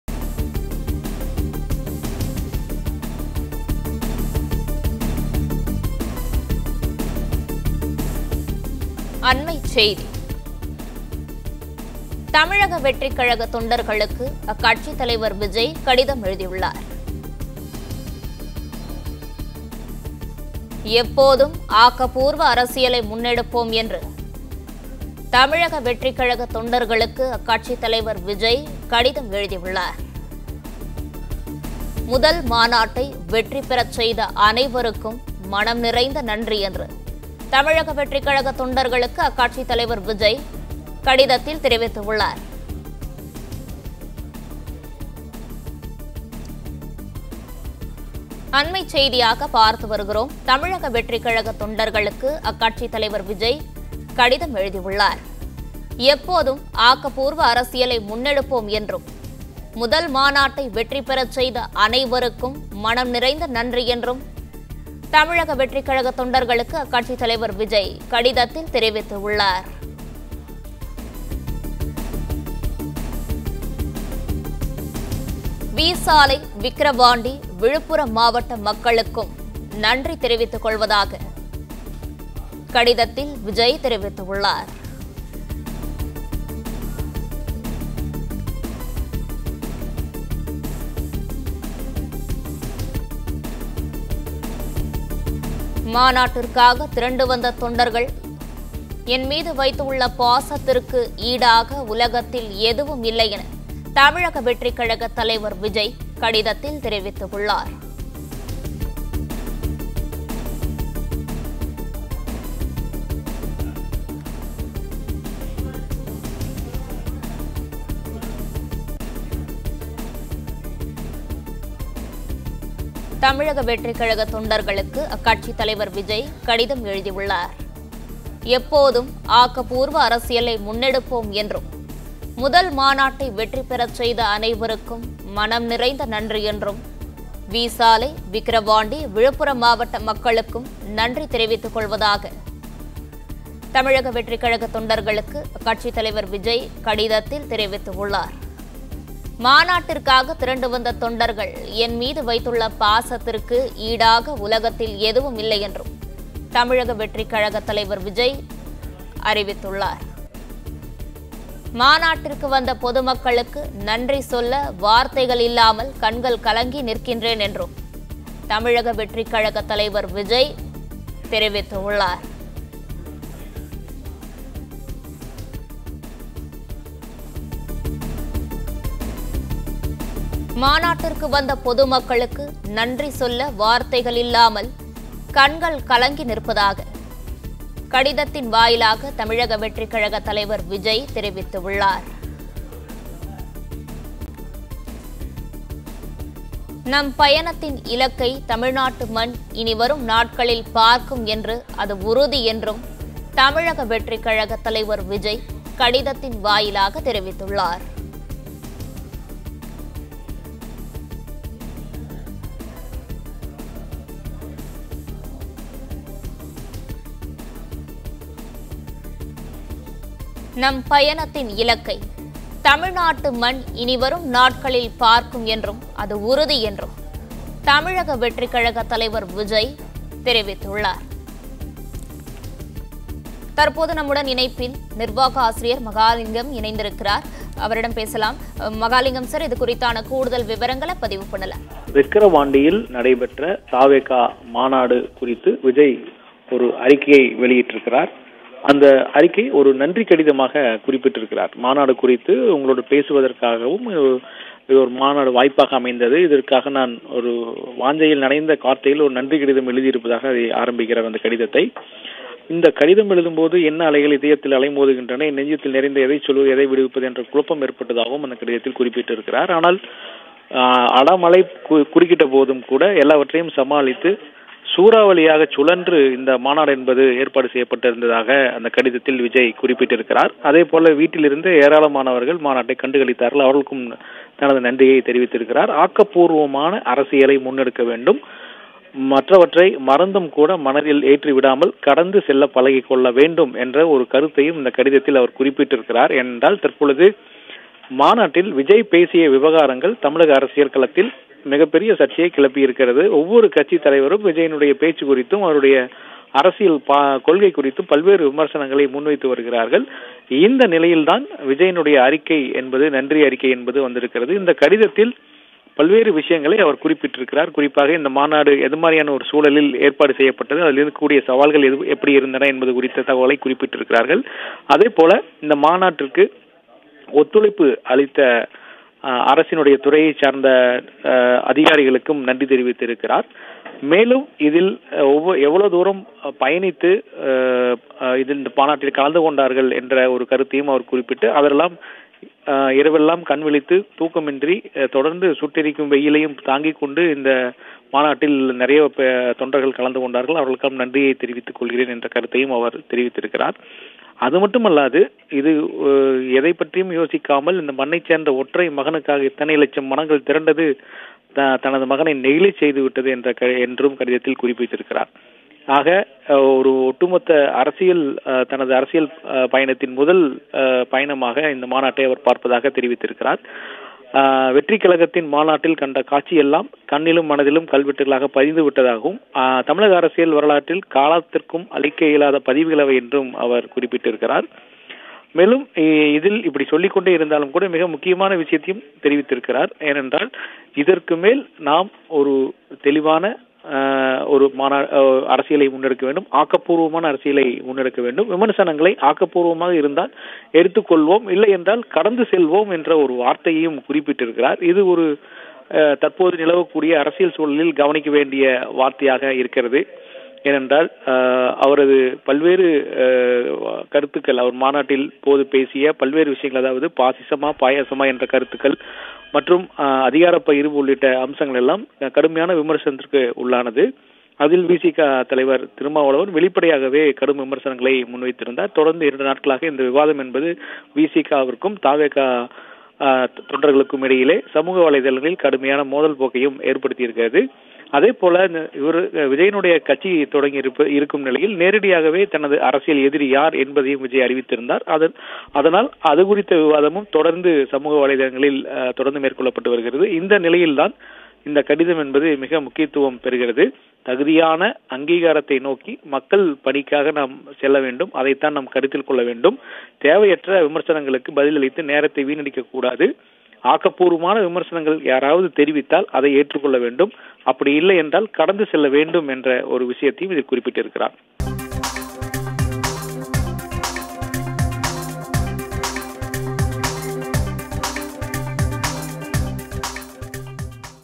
BREAKING || மாநாடு அமோக வெற்றி.. பற்றி எரியும் அரசியல் - தொண்டர்களுக்கு விஜய் ட்வீட்..!!
JustNow || மாநாட்டிற்காக திரண்டு வந்த தொண்டர்கள் என் மீது வைத்துள்ள பாசத்திற்கு ஈடாக உலகத்தில் எதுவுமில்லை - விஜய்..!!